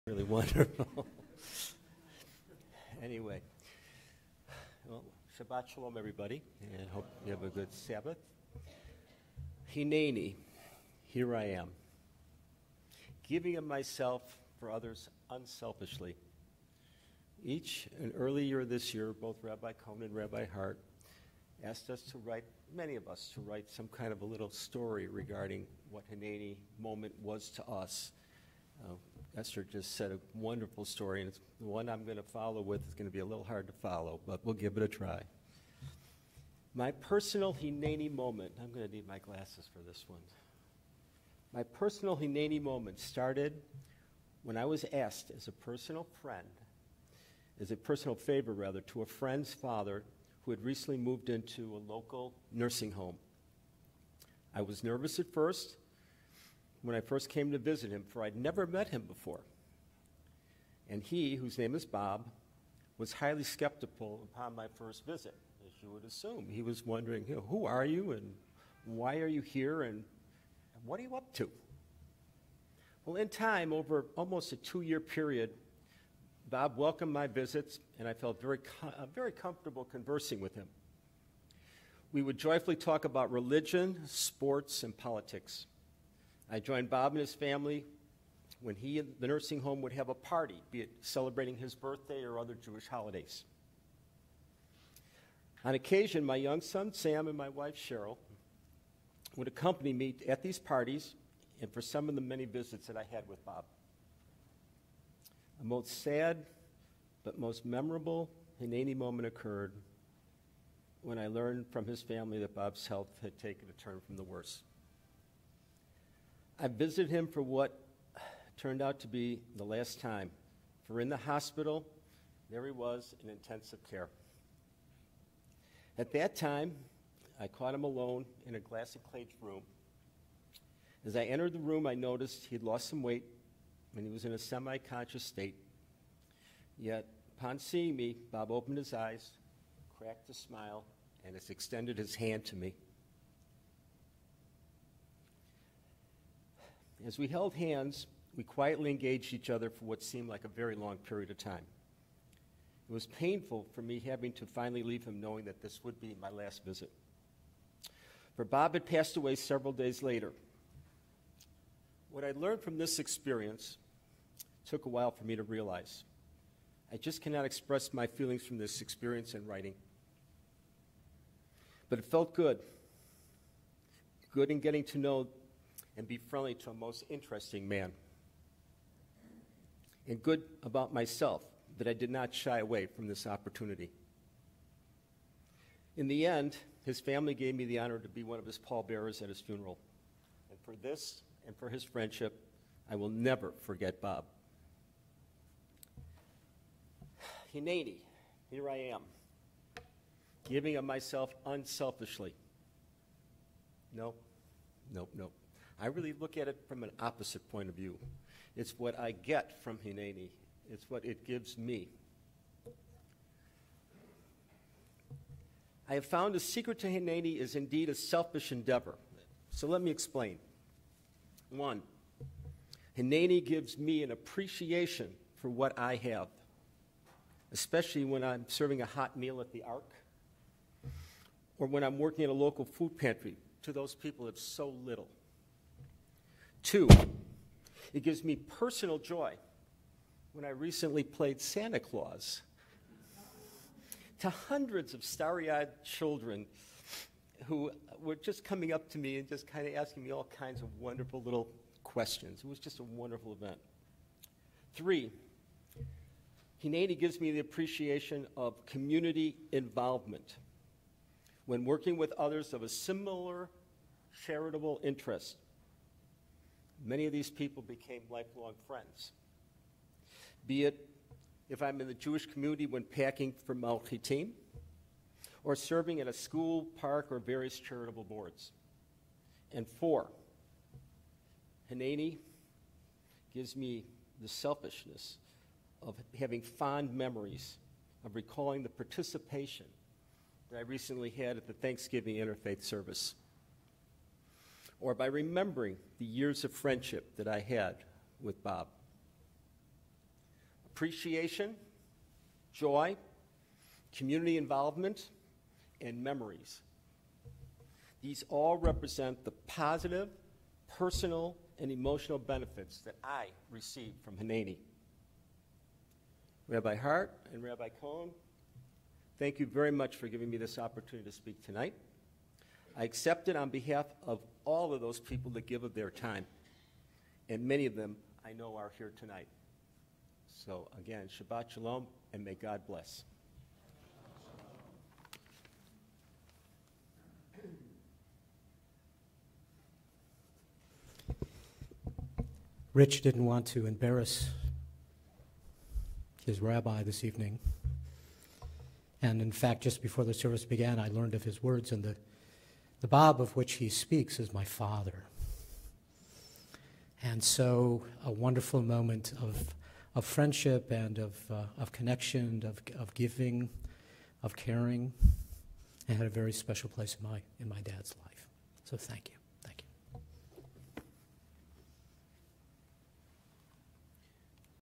Shabbat with Joy & Presence - Richard Rosen
Richard Rosen, Northwest Suburban Interfaith Council President, shares his "Hineini" story at Kabbalat Shabbat worship at Temple Chai Long Grove, IL.